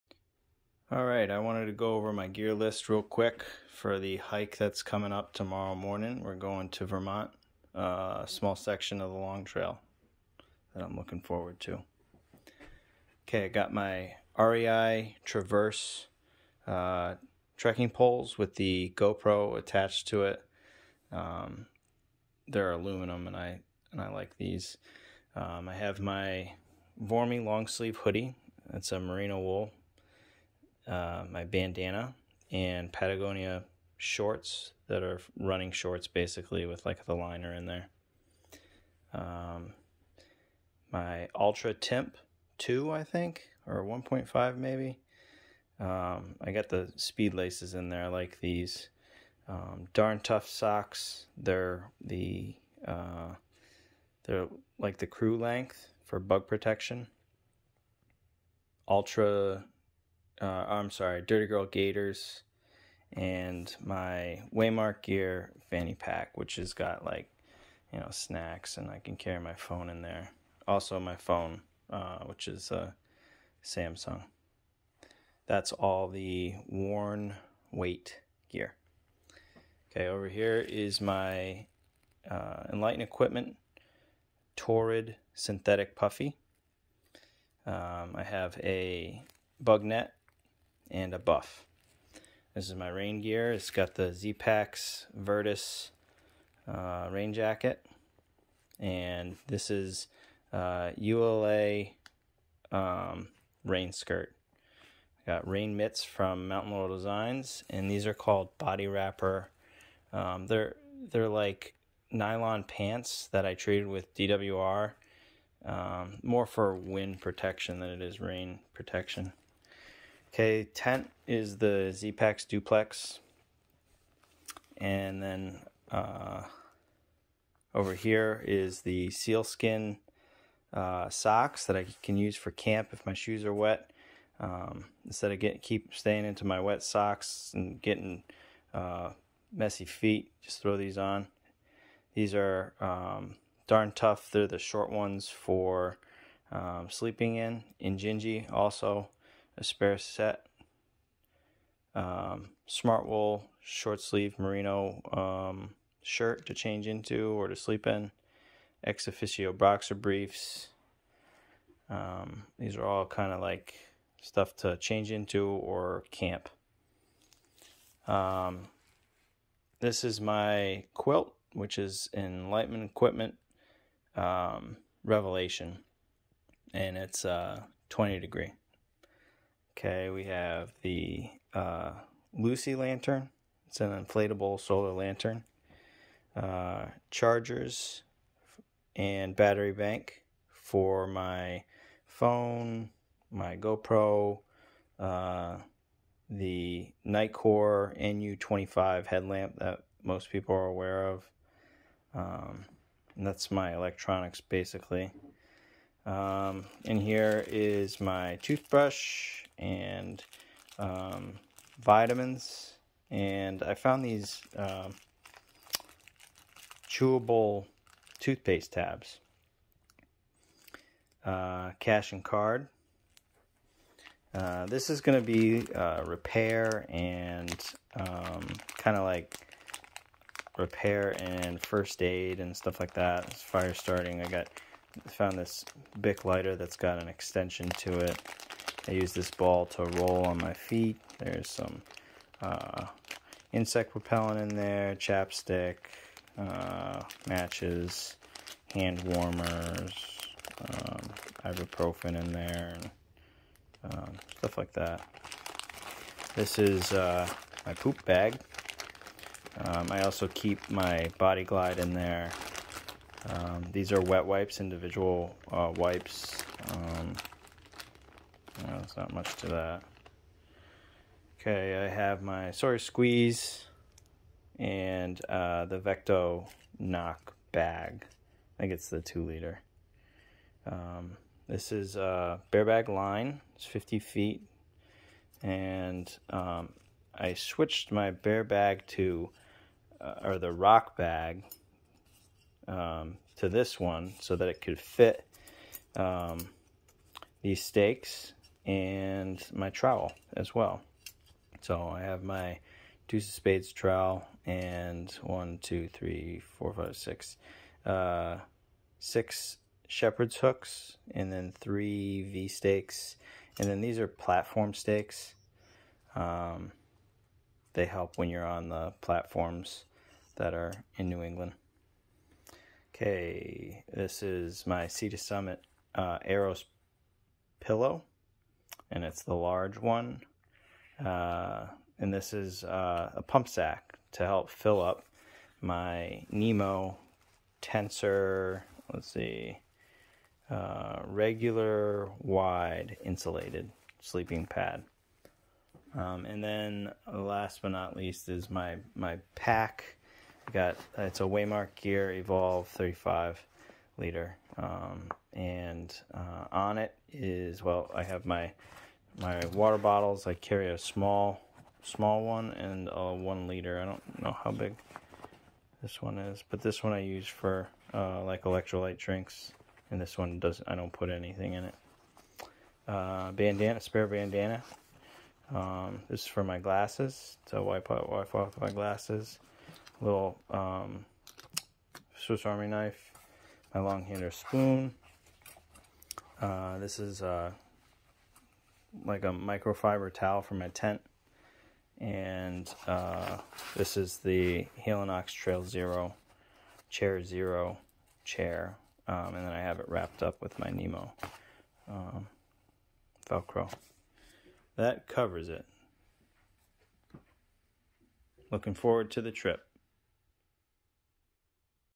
Vermont Long Trail September 2021
A gear list for Stratton pond Bromley area of the VT Long Trail. We left on 9/9/21 and returned on 9/12/21. One day we expected a small amount of rain. We had a resupply spot we left food and dry clothes to change into about half way. Many thanks to my friend Lee for planning this trip.